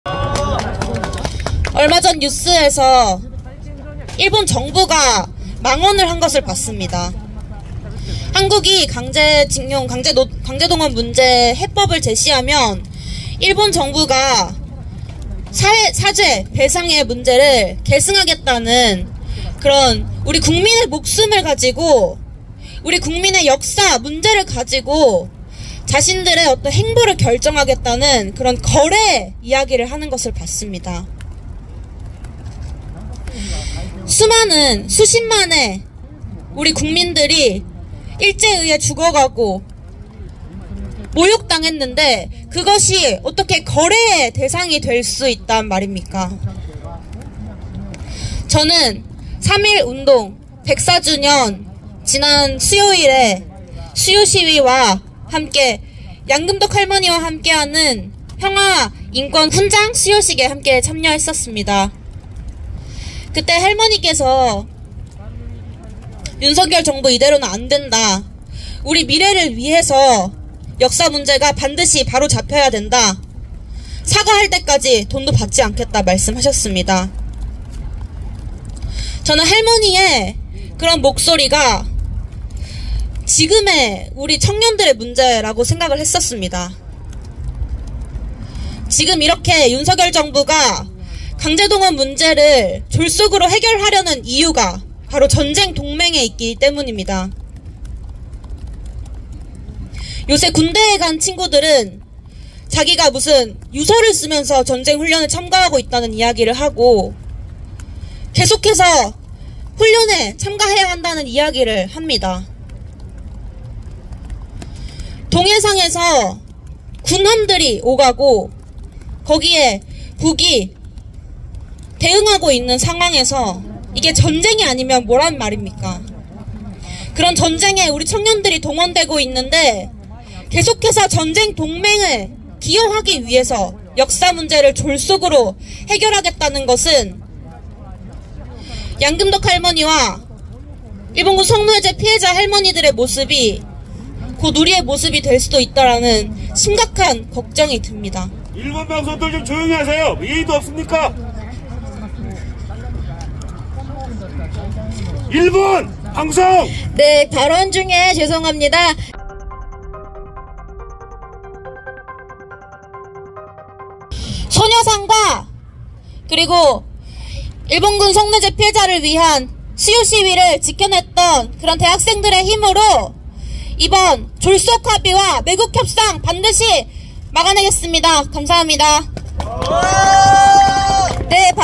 윤석열 일제 강제 동원 굴욕 해법 강행 규탄 기자회견 - 강새봄 진보대학생넷 대표 @외교부 청사 앞(2023.03.06.)
"면담 어렵다”는 강제동원 103살 피해자 집 불쑥 찾아간 외교부

윤석열 일제 강제 동원 굴욕 해법 강행 규탄 기자회견
일시: 2023년 3월 6일
장소: 외교부 청사 앞
주최: 한일역사정의평화행동

(발언 내용은 채널 전체 방향성과 일치하지 않을 수 있습니다)

▶[구독]과 [좋아요], [가입], [SuperThanks], [후원]은 방송 제작에 큰 힘이 됩니다◀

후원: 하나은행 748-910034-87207